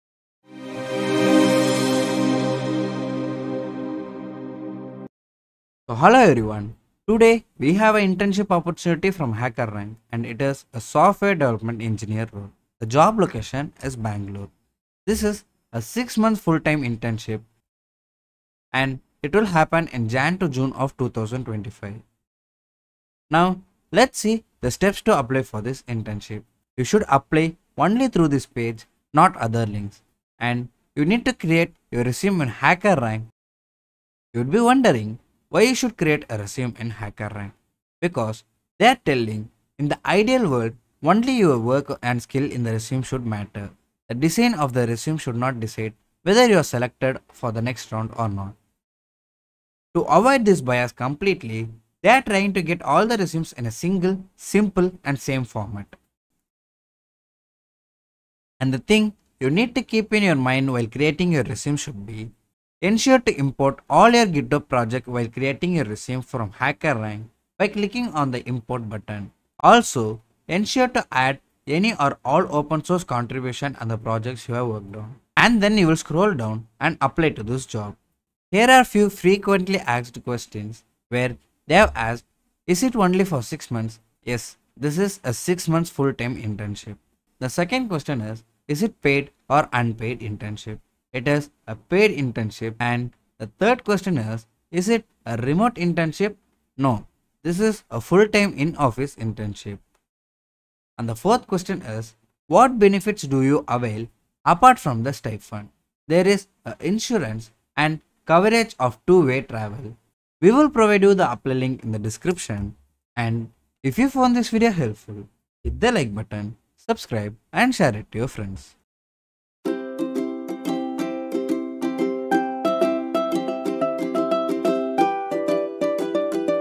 HackerRank Software Development Engineer Internship 2025: Unlock Your Future in Tech
Are you a passionate coder eager to jumpstart your career? HackerRank is offering a 6-month Software Development Engineer Internship in Bangalore from January to June 2025! This full-time onsite opportunity is perfect for graduates ready to immerse themselves in real-world projects and learn from world-class mentors.

What to Expect:

Hands-on Experience: Write impactful code and contribute to meaningful projects from day one!
Mentorship: Pair with experienced mentors who will guide you throughout your internship.
Transparent Culture: Gain insights into company progress directly from the executive team and enjoy open communication across the organization.
Career Growth: Potential for a full-time offer upon successful completion of your internship.

FAQ Highlights:

Duration: 6 months, onsite in Bangalore.
Compensation: This is a paid internship with additional benefits.
Join us at HackerRank and take the first step towards an exciting tech career!